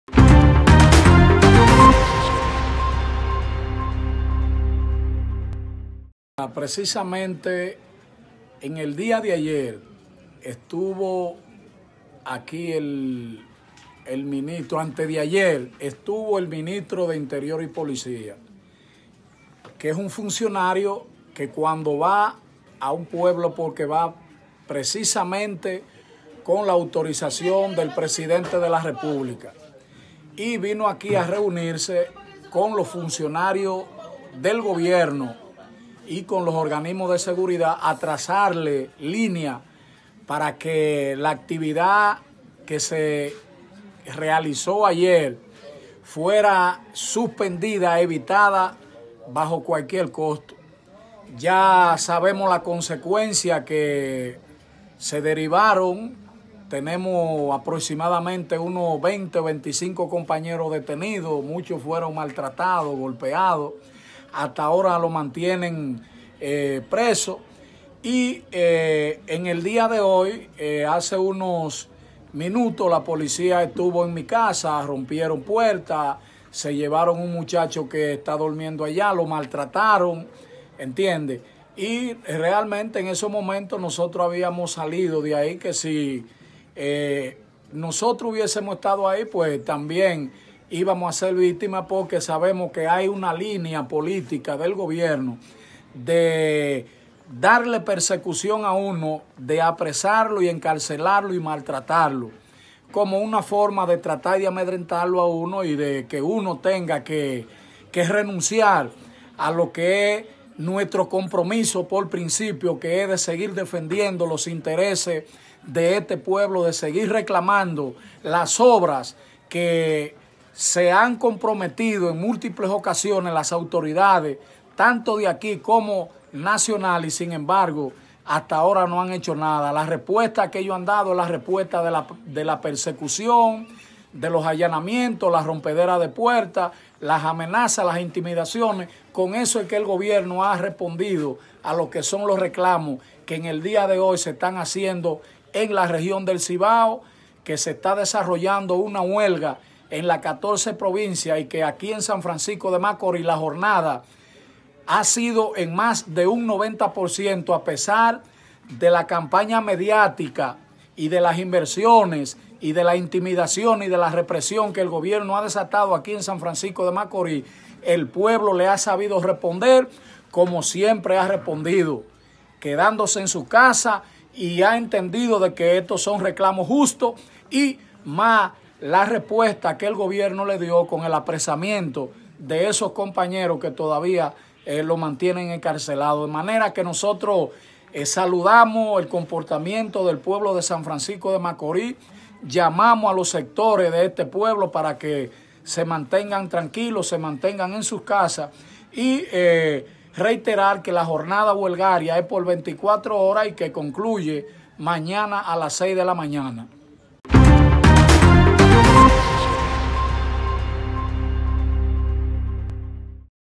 Continúan detenidos más de 20 convocantes a huelga en SFM; Aseguran gobierno intenta reprimirlos
San Francisco de Macorís.- El dirigente comunitario y popular Ángel Reyes, conocido popularmente en esa ciudad como Camarada, dijo que hasta el momento más de 20 miembros del movimiento continúan detenidos,  los cuales fueron apresados previo al inicio de la huelga.

De acuerdo con las declaraciones de Camarada, quien también fue apresado ayer y puesto en libertad, El ministro de interior y policía visitó esta ciudad antes ayer, en procura de que la marcha en la cual fueron apresada ma veintena de dirigentes, no se llevará acabo.

Denunció además que algunos de los detenidos fueron golpeados y maltratados. Informó que en el transcurso de la mañana de este lunes, agentes de la policía nacional interrumpieron en su casa, rompiendo puertas y deteniendo a un joven que allí estaba, presume que con el objetivo fue de amedrentar a los constantes.

Reiteró que la huelga se va a llevar a cabo por 24 horas en esa ciudad al igual que las demás ciudades que componen la región del Cibao hasta mañana martes a las seis de la mañana.